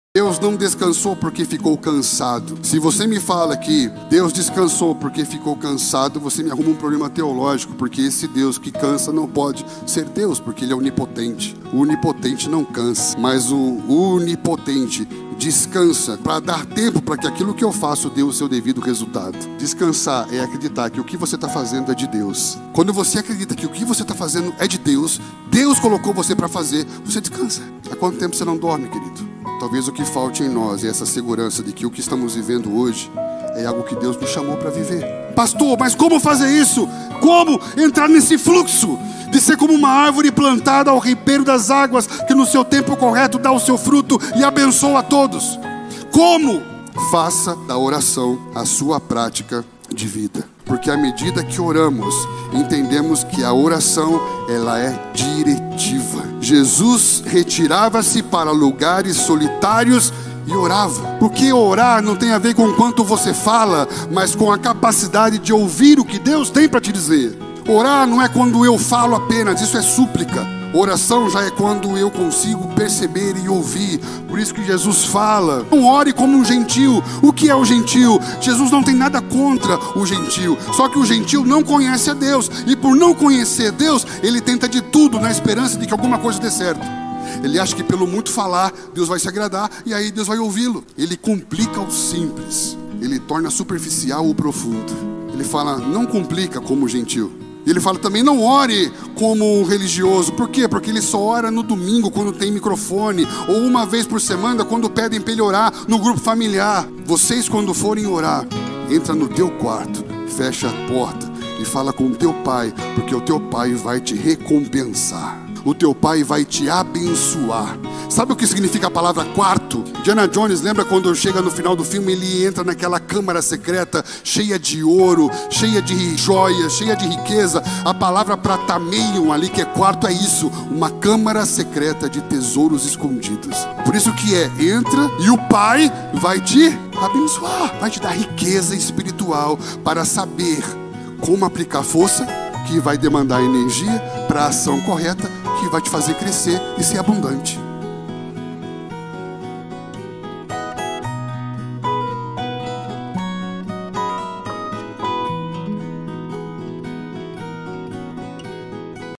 ENCONTRE O FLUXO da VIDA PRODUTIVA e DESCUBRA o VERDADE sobre o PRINCÍPIO DO DESCANSO...
O PRINCÍPIO DO DESCANSO
Compartilhando recorte da mensagem ministrada domingo dia 21 de janeiro na @igrejafamiliaonline sobre três princípio, baseado no livro: 20 princípios de Jesus Cristo para uma vida abundante.